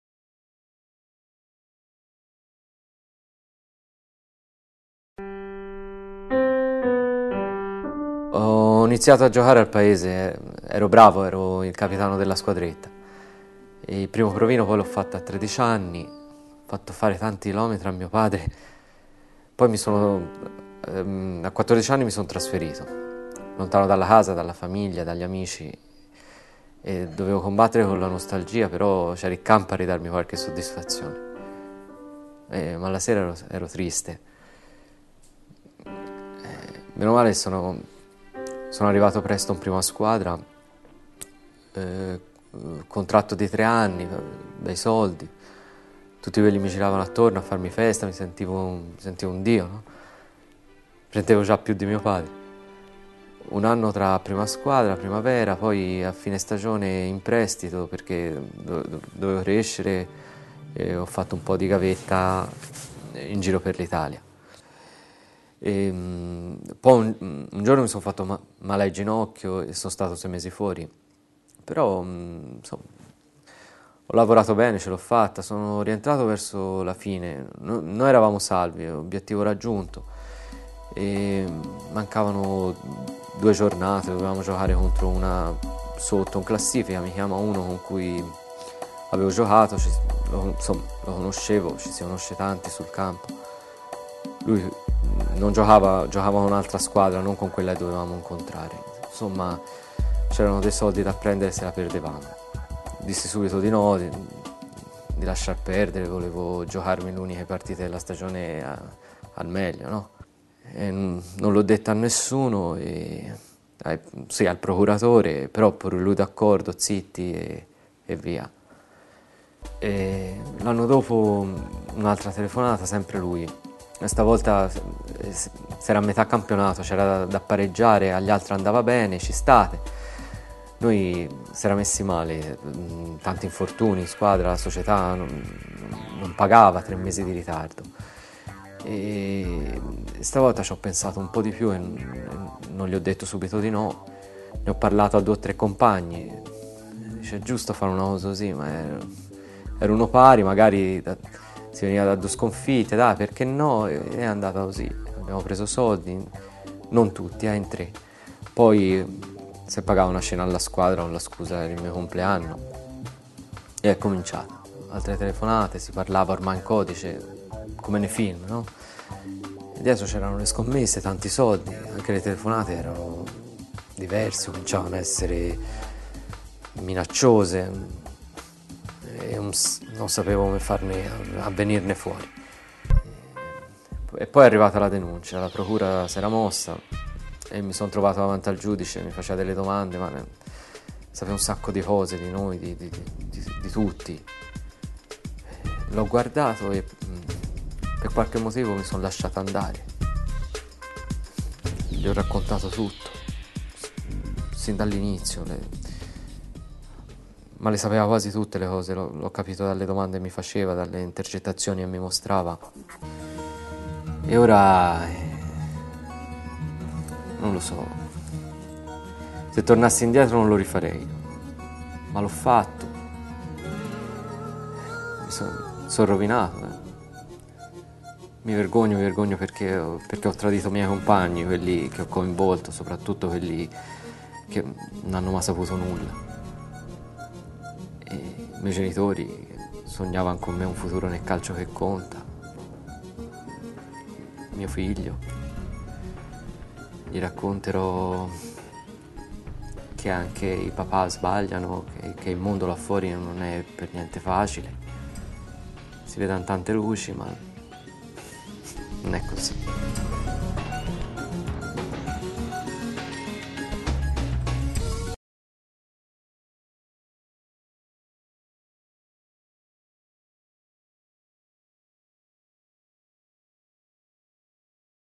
Calcioscommesse: giocatore pentito racconta la sua storia
Nel video dell'Assocalciatori un giocatore pentito racconta come è entrato nel vortice del calcioscommesse. Per maggiori info su